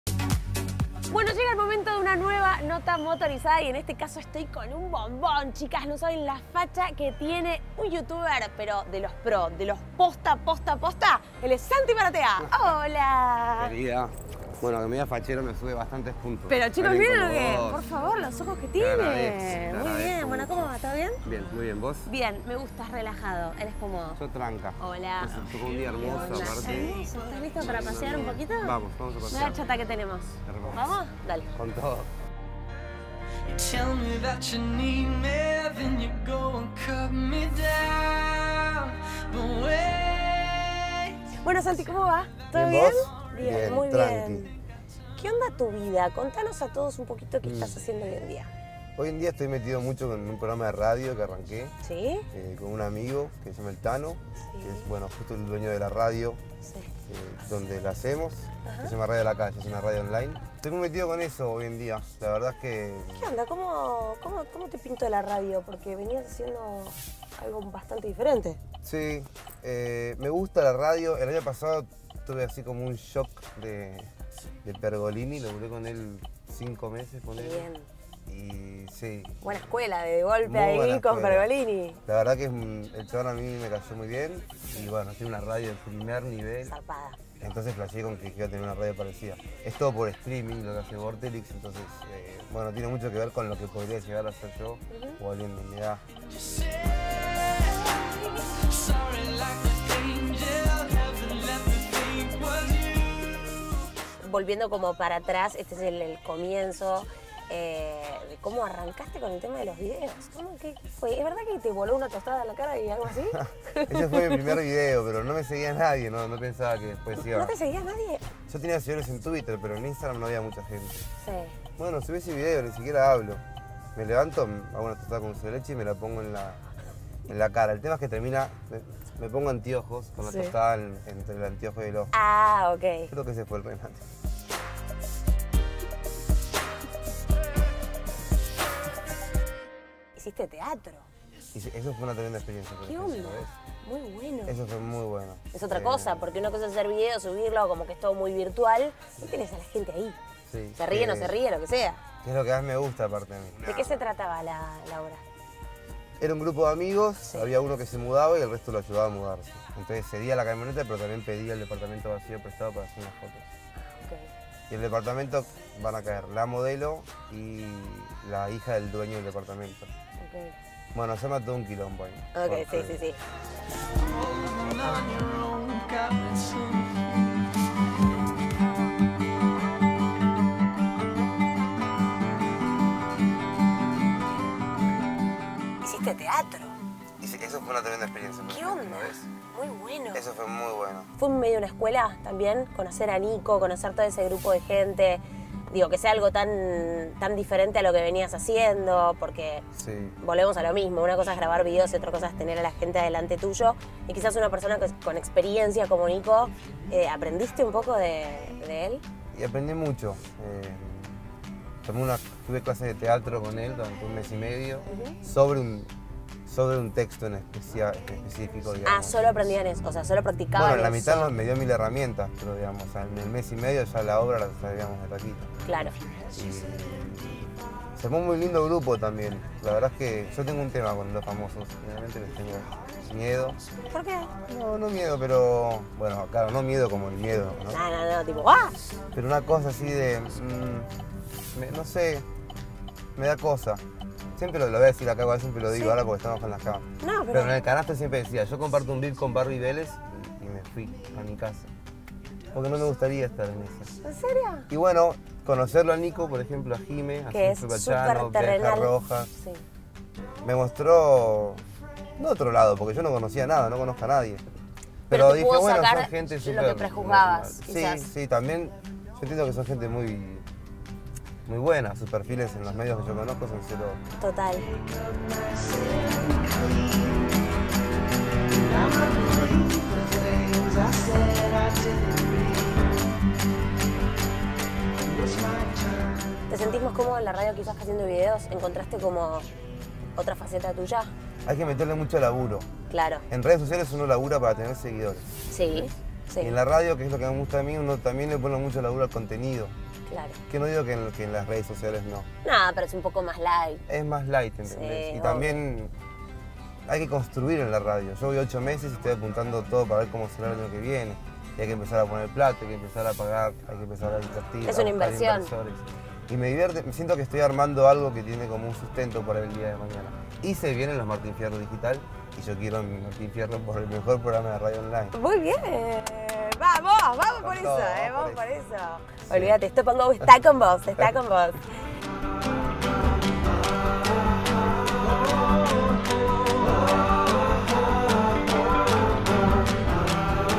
Santi Maratea "Los Famosos ME DAN COSA"
Ivi Nadal entrevista a Santi Maratea